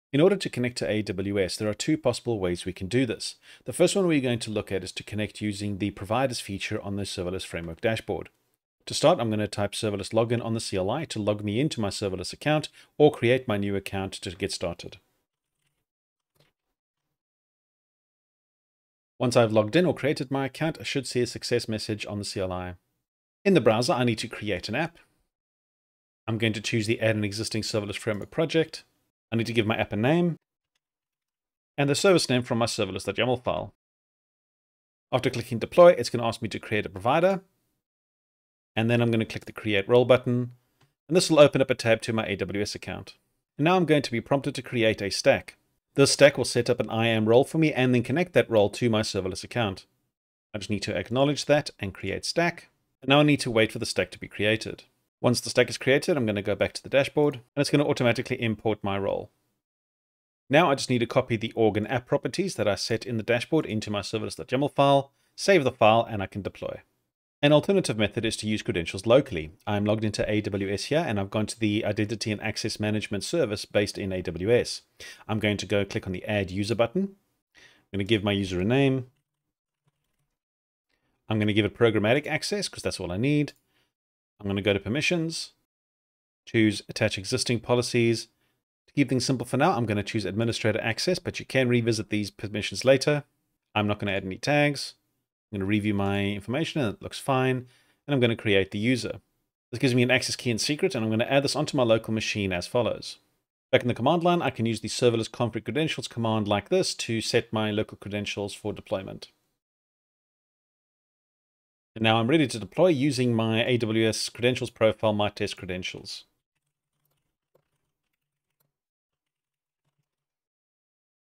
Connecting to AWS
Find out how to connect your Serverless Framework deployment to your AWS using Providers from your Serverless Dashboard account or local credentials with an IAM Access Key and Secret.